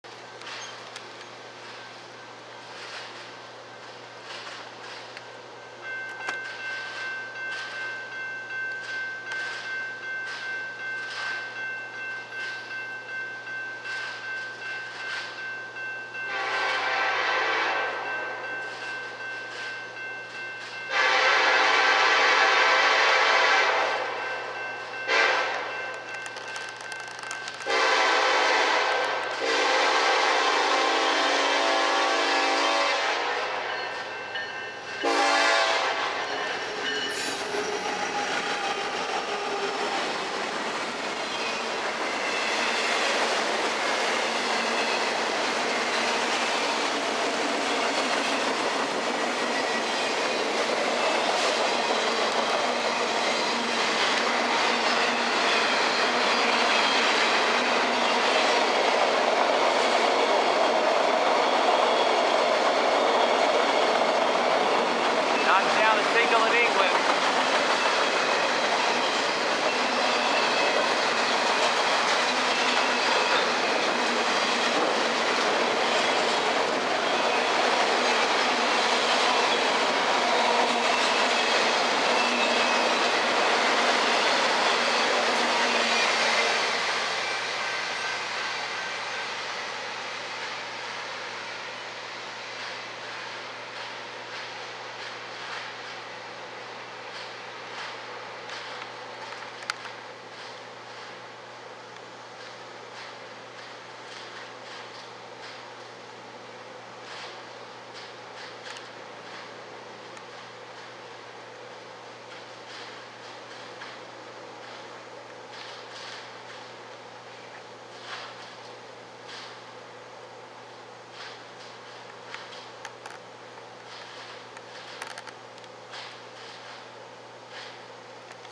Norfolk Southern 284 NB Autoracks w/ ACe & K5LLA! Austell,Ga 07-25-2015©
8:18PM in Austell,Ga is NS 284 Northbound autoracks with SD70ACe leading!!

NS 8922 Dash 9

Recorded by Gene and Tommy Hale on July 25, 2015